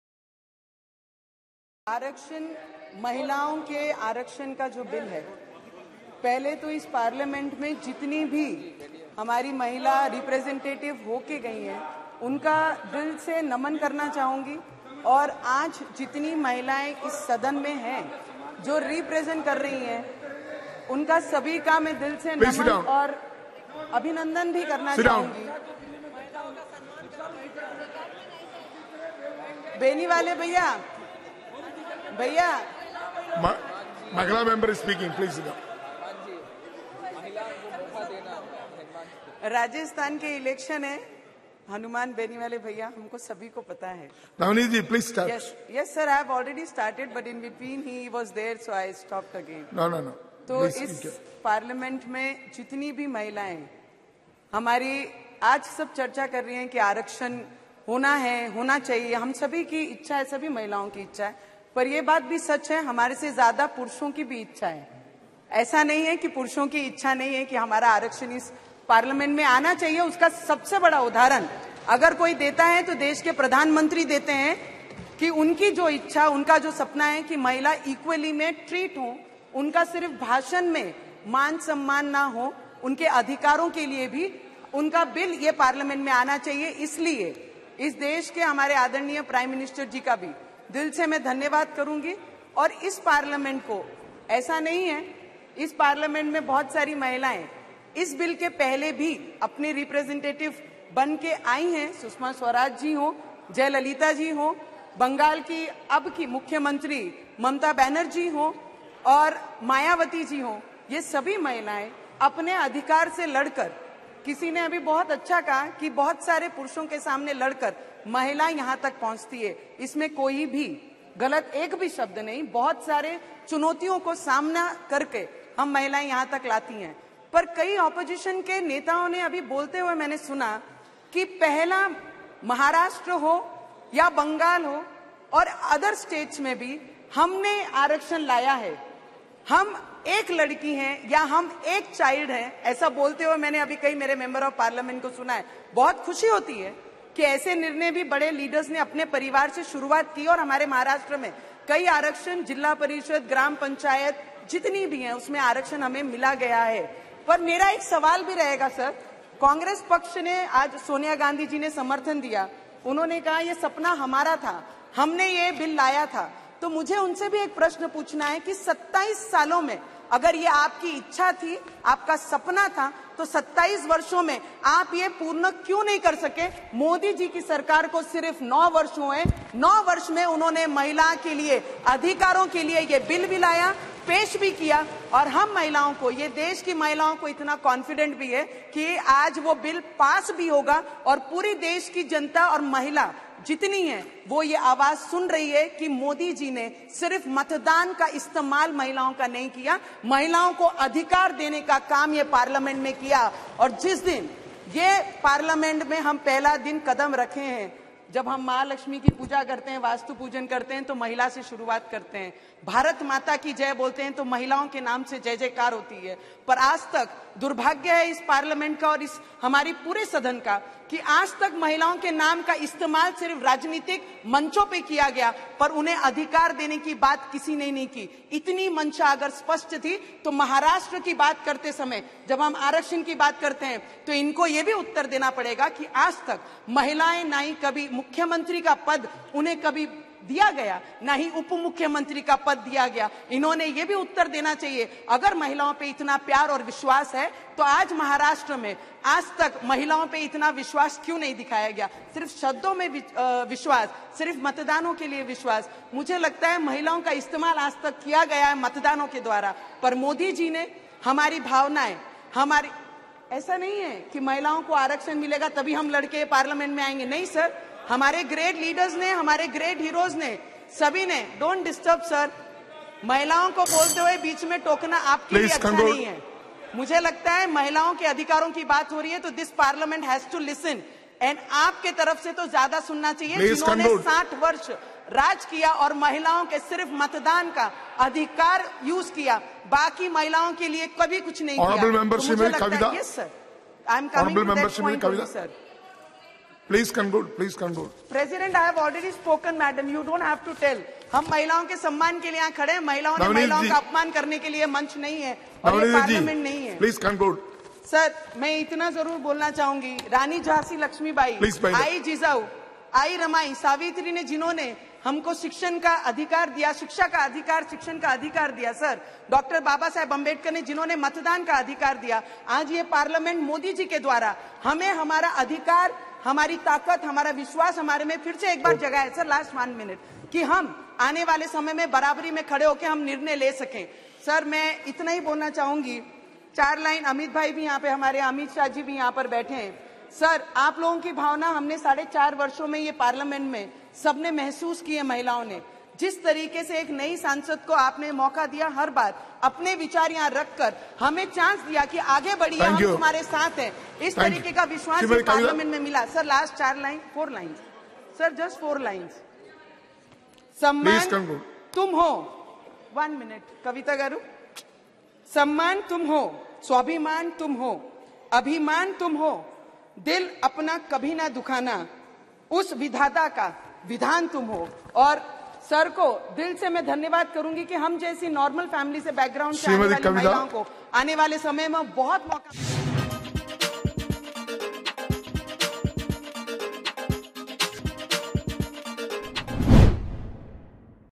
Navneet Rana on Women Reservation : 'महिलाओं का इस्तेमाल मतदान के लिए किया गया...'
अमरावती से निर्दलीय सांसद नवनीत राणा ने महिला बिल का समर्तन करते हुए विपक्ष पर जोरदार हमला बोला। नवनीत राणा ने कहा कि विपक्ष ने आजतक सिर्फ महिलाओं पर मतदान के लिए विश्ववास दिखाया, हमारे हित के बारे में नहीं सोचा। आजतक केवल महिलाओं का इस्तेमाल मतदान के लिए किया गया। आज पीएम मोदी ने हमपर विश्वास जताया।